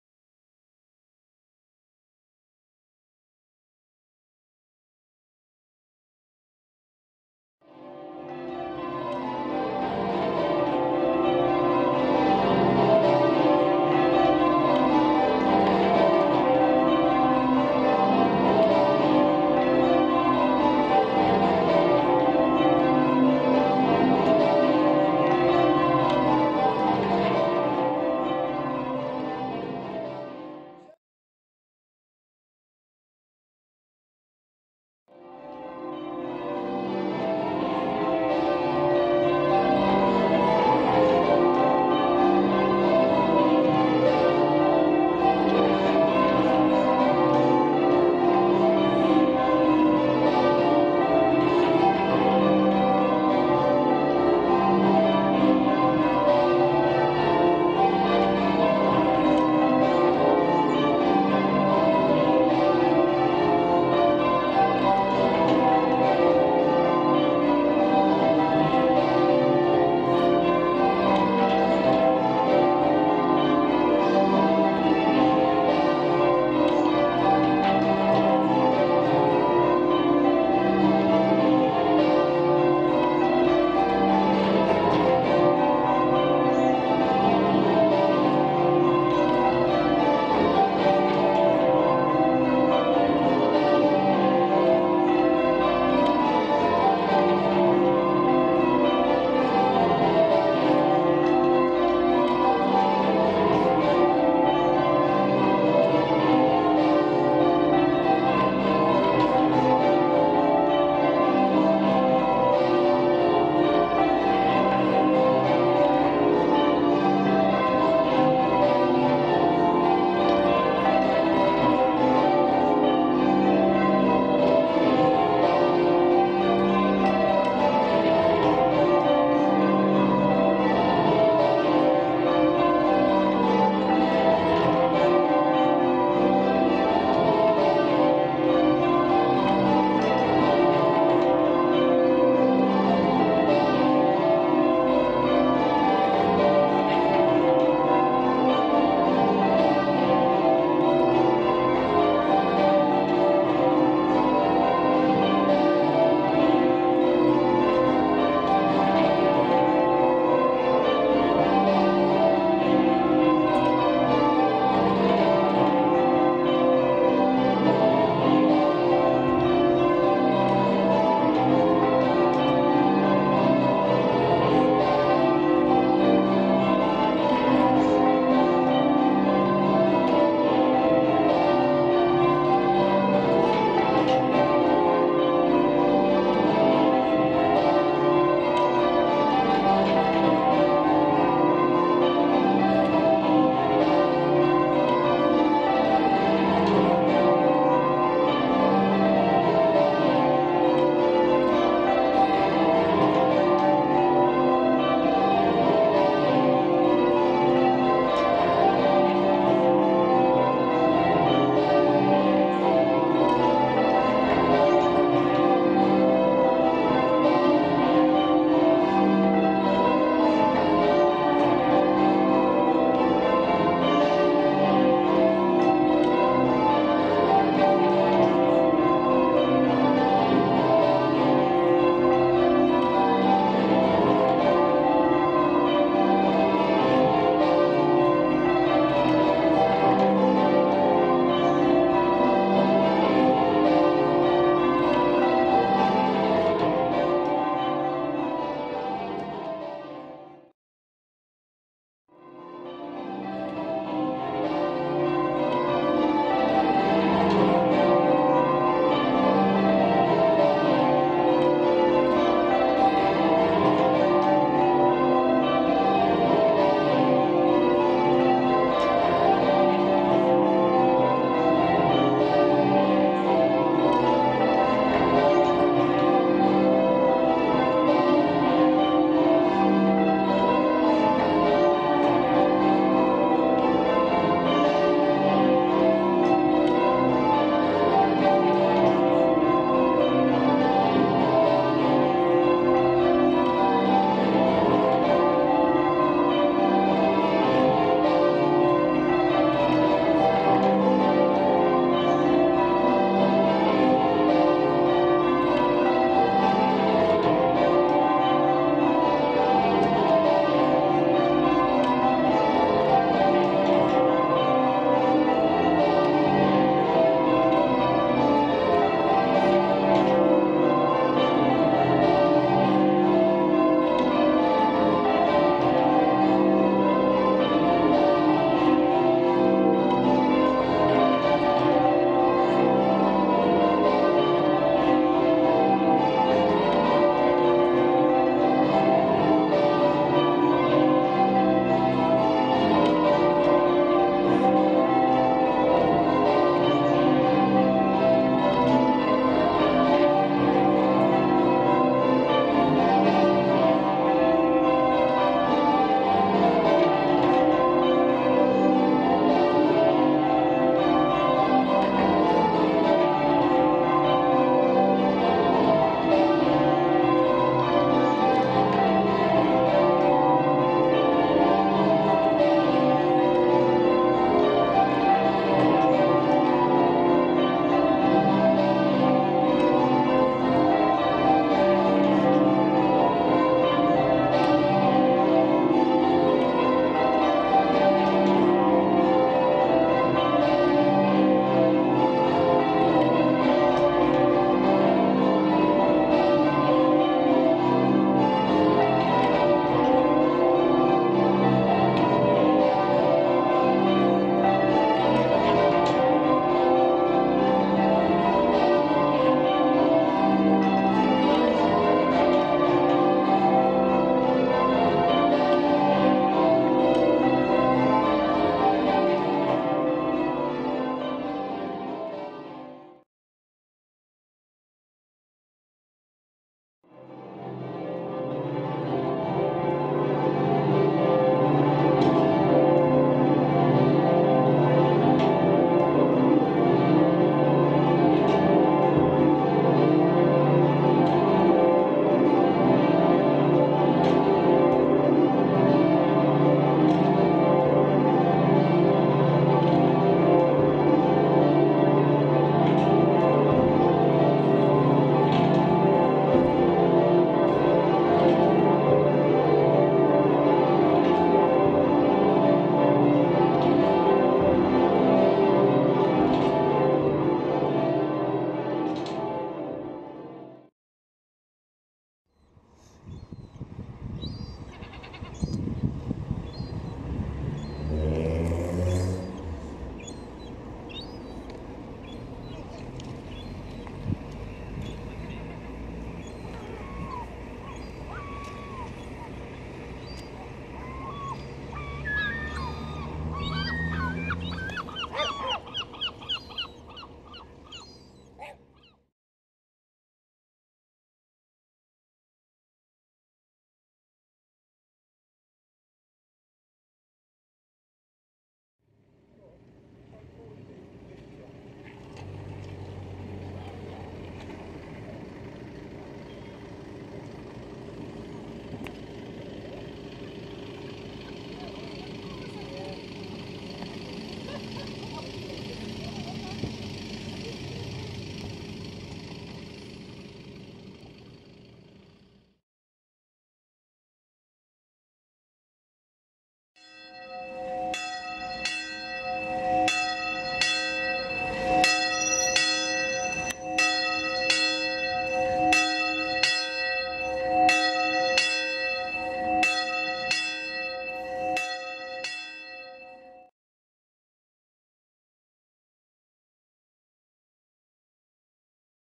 Bristol Surprise Royal at Inveraray, Argyll and Bute
A couple of extracts from a peal on the, quite simply, magnificent bells of Inveraray, Scotland – the second heaviest ring of ten, with a tenor weight of 41-2-8, tuned to C.  They can be described very simply – HO HO HO!

At the end of the First World War, the bells and detached tower were commissioned by Niall Diarmid Campbell, the 10th Duke of Argyll, as a war memorial to the Clan Campbell fallen.  Work began on the tower in 1921, and was not completed until 1931; the bells had already been cast by Taylor’s of Loughborough in 1920! The bells were housed in a wooden shed behind the church until completion of the tower, and chimed via a keyboard before finally being hung in September 1931. The “bell cage” still survives, but there isn’t a lot left inside it.

The detached Bell Tower rises to a height of 126 feet with 176 stairs from top to bottom. With its Somerset-like appearance, it dominates the landscape; a very lasting memorial!  It was always intended to join the tower to the church, but a lack of funds, followed by the outbreak of the Second World War, prevented this from ever happening.  The tower was damaged during the war, and became derelict until the 1970s, when a huge amount of restoration work took place, led by the late Norman Chaddock, to create the tower “visitor experience” and sort the bells out (including hefty sound control, as the bells thundered out of the huge sound windows!).

They are a truly superb ring of bells in every way; they handle well and sound amazing, with much work having been done in recent times to improve the acoustics.  They are well looked-after, and it was simply a pleasure to ring here.   The peal, whilst a bit on the slow side at the beginning, was very enjoyable indeed, with a suitably interesting and musical composition (as expected!) from Alan.

As well as the wonderful bells, housed in the detached Bell Tower, the town itself, nestled on Loch Fyne, boasts some stunning scenery; the view I had from the ringing chamber window was second to none!

After a short clip of the lower on the back 8, I finish with a shot of the “other” bell in the town!  Hung in the south facade of the Glenaray and Inveraray Parish Church, it was re-cast in 1728 by Robert Maxwell of Edinburgh.  This building, which more resembles a town hall than a church, used to have a slender spire; this was deemed unsafe and taken down, stone by stone, in November 1941, during WW2.  Each stone was numbered for re-erection at a later date, and the stones were stored in the local quarry – never to be seen again!